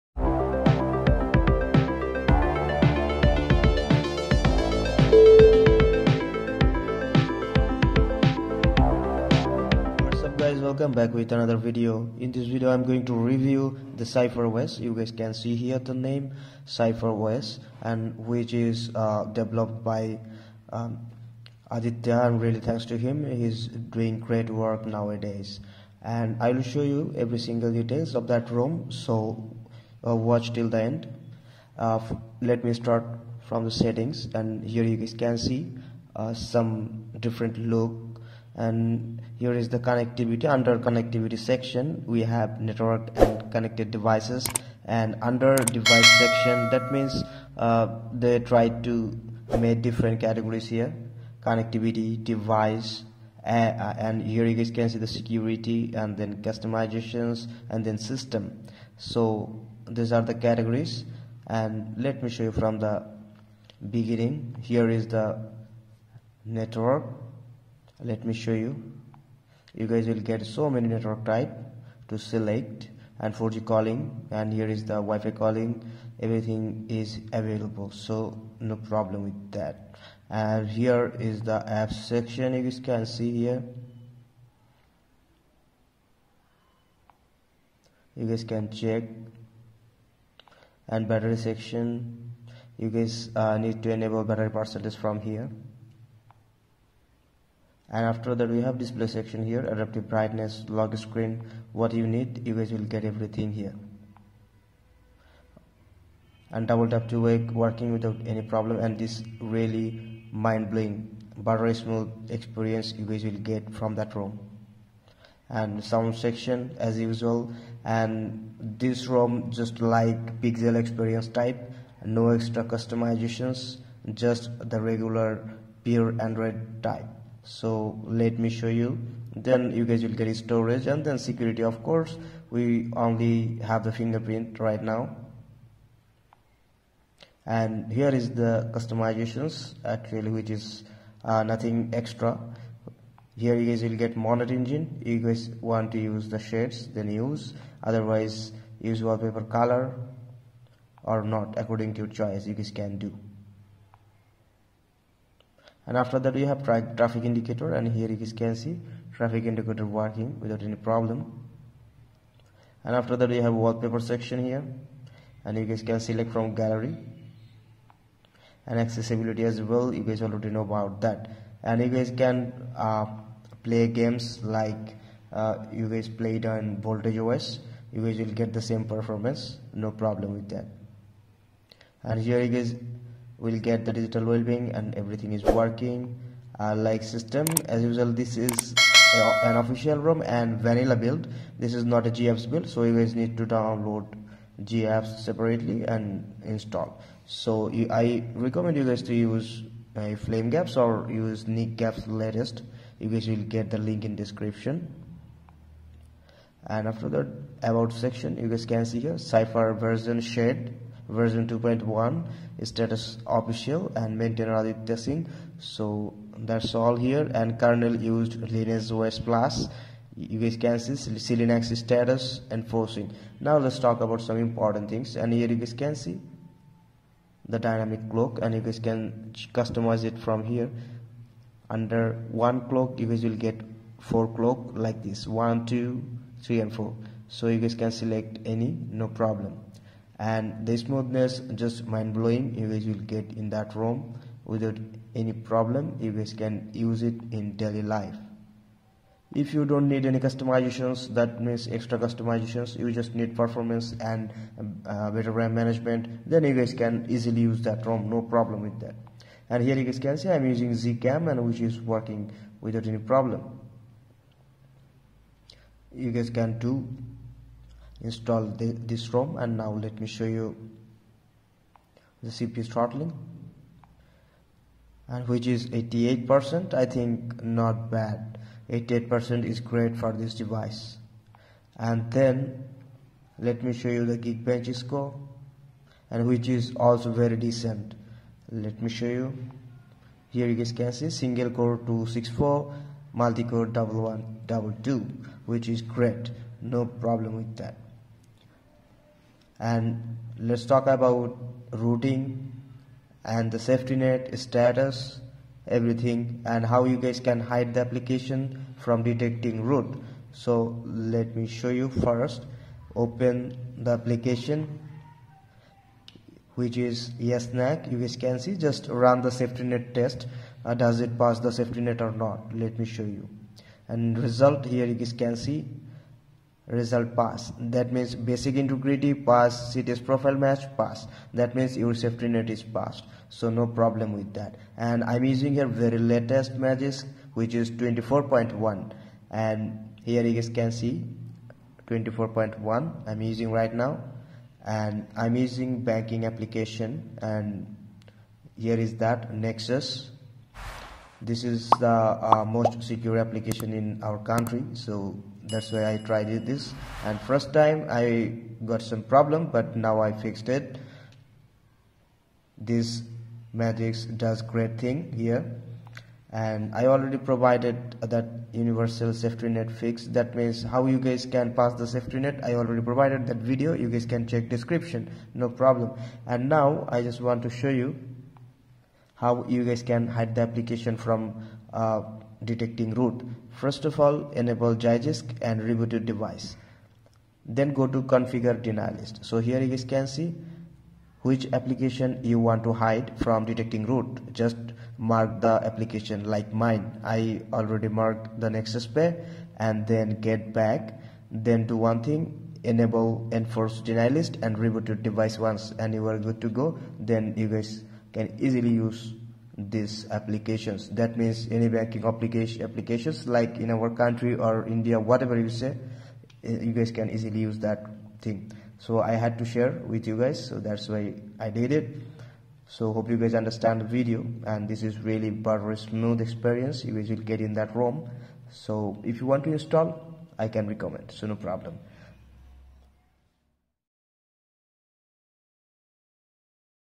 Cipher OS Android 12 Rom For Redmi 7/Y3|Smooth Experience Daily Drive Rom|Must Watch For Magisk Hide
In This video i will review Cipher OS For Redmi 7 and redmi y3.Install Cipher os android 12 in redmi 7 and redmi y3.Another best Custom rom like pixel experience.

Credit : ADITYA
Recent Videos